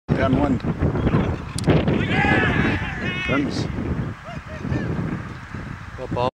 Bowled em....
A happy Luke Scully after Fletch Te Aika lets rip with a yorker against the Cairns Invitational team.
5 Oct 2010